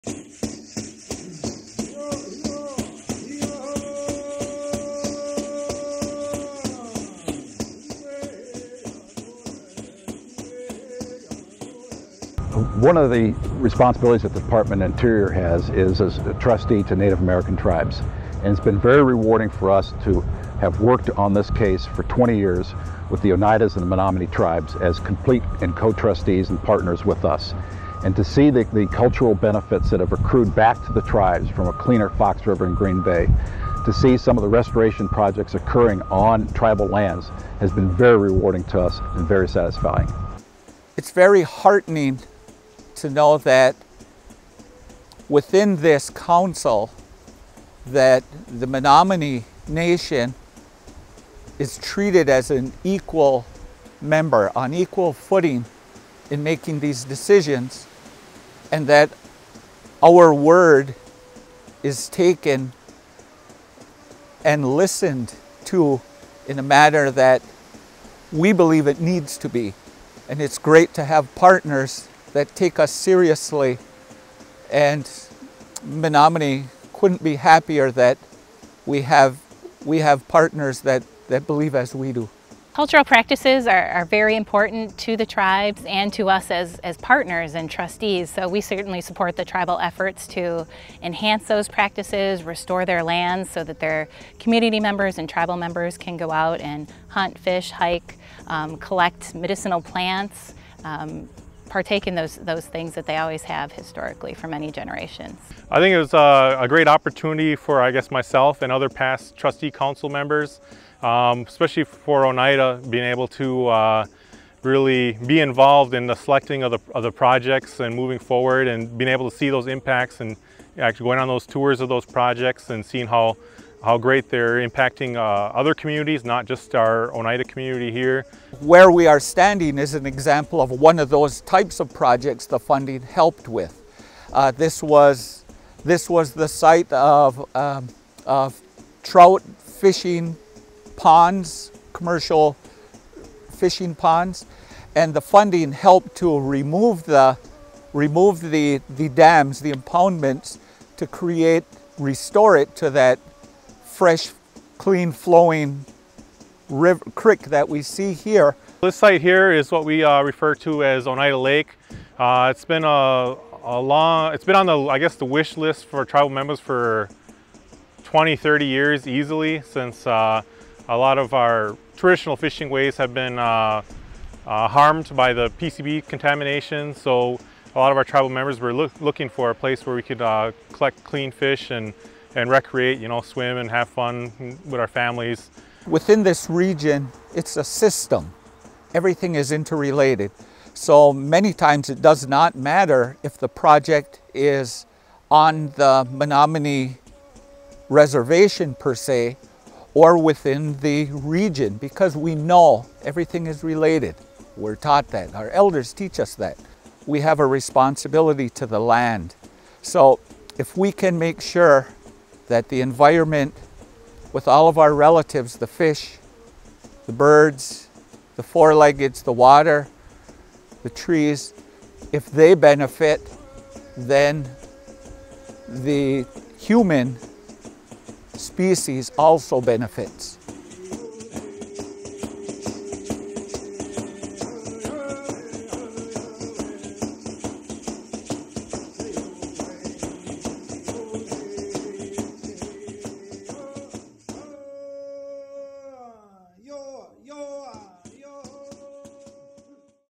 Fox River/Green Bay NRDA - Cultural Involvement
Through the Natural Resource Damage Assessment (NRDA) Process, the Fox River/Green Bay Trustees have implemented a wide variety of projects that restore, rehabilitate, replace or acquire the equivalent of natural resources that were injured as a result of contamination-the release of PCBs in the Fox River-and will continue to do so into the future.
This video highlights the involvement of the Menominee and Oneida Tribes in the NRDA process and the focus on restoring cultural resources.